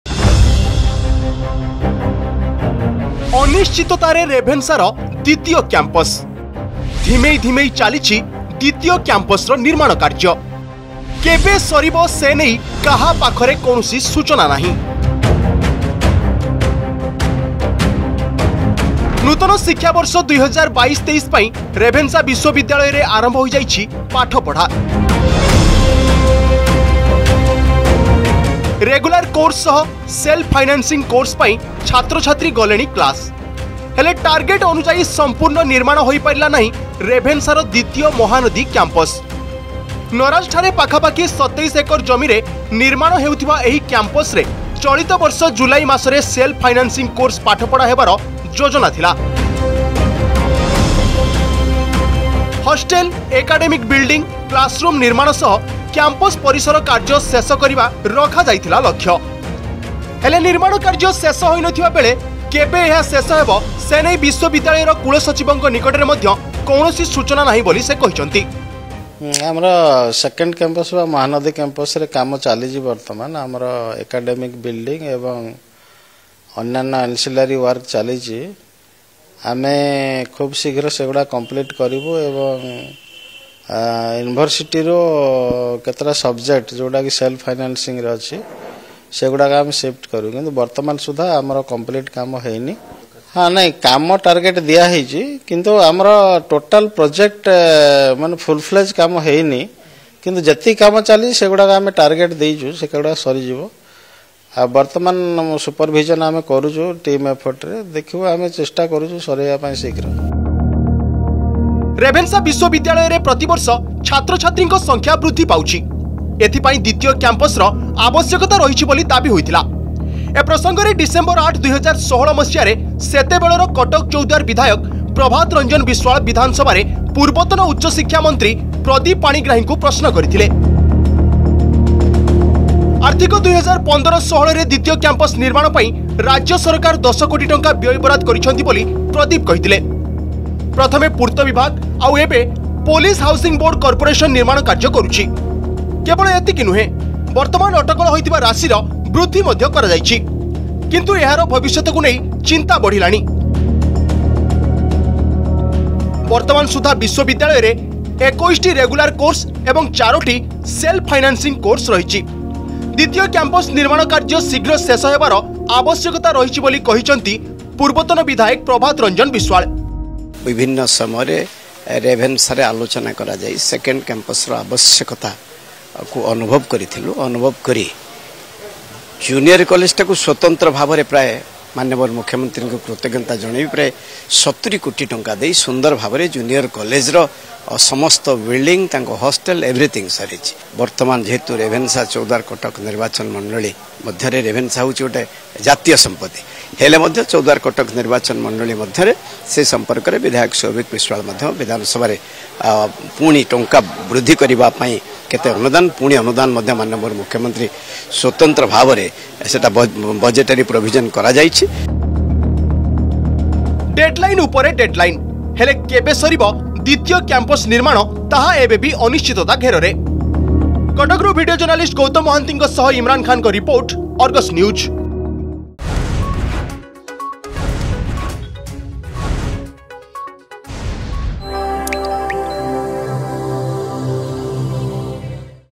ଅନିଶ୍ଚିତତା ଭିତରେ ରେଭେନ୍ସାର ଦ୍ଵିତୀୟ କ୍ୟାମ୍ପସ
ଧିମେଇ ଧିମେଇ ଚାଲିଛି ନିର୍ମାଣ କାର୍ଯ୍ୟ । ନରାଜ ଠାରେ ୧୨୭ ଏକର ଜମିରେ ଭିତ୍ତିପ୍ରସ୍ତର ସ୍ଥାପନ କରିଥିଲେ ମୁଖ୍ୟମନ୍ତ୍ରୀ  । କେବେ ସରିବ ଦ୍ବିତୀୟ କ୍ୟାମ୍ପସ ନିର୍ମାଣ କାମ ?